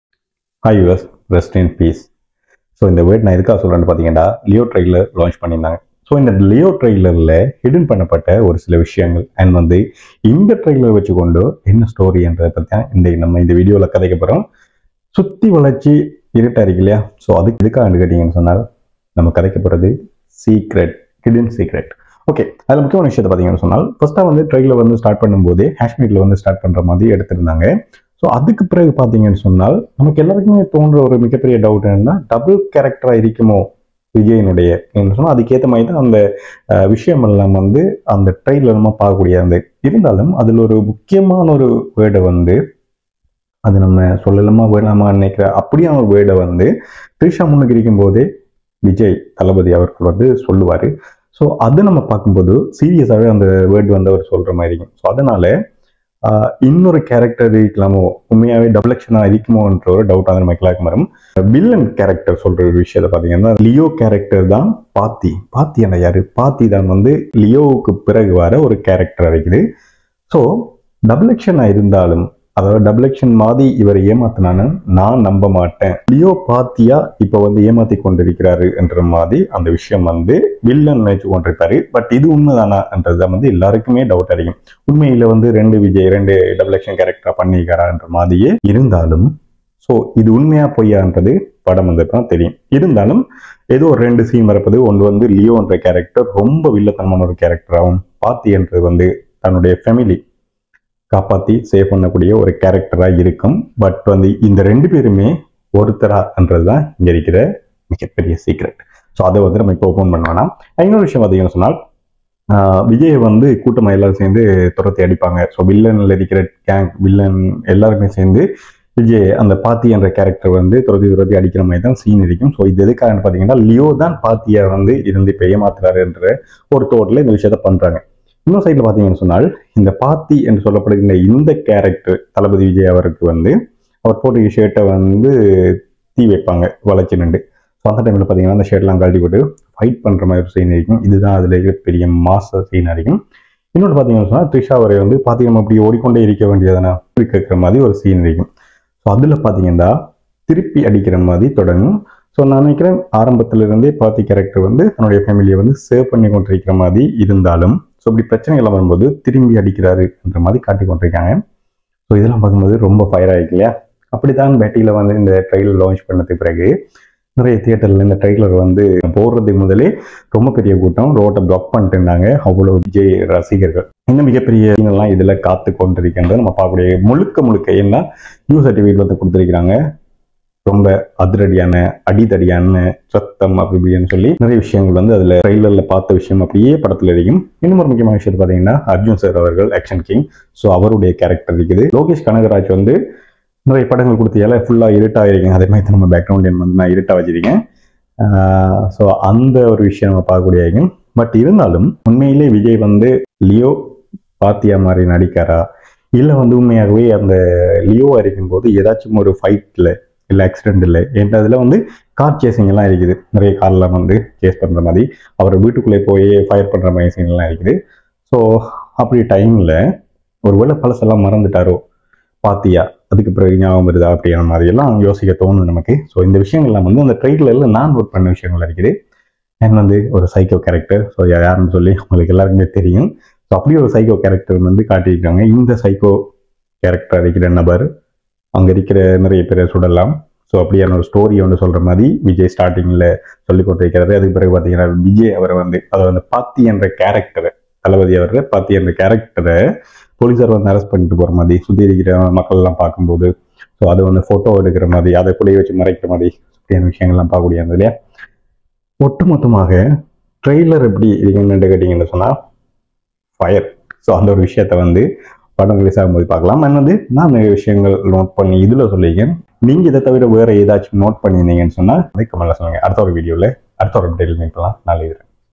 LEO - Trailer | Hidden Story | Vijay | Lokesh | Anirudh | Tamil | Lihi Tharan
LEO Trailer Hidden Secrets Explained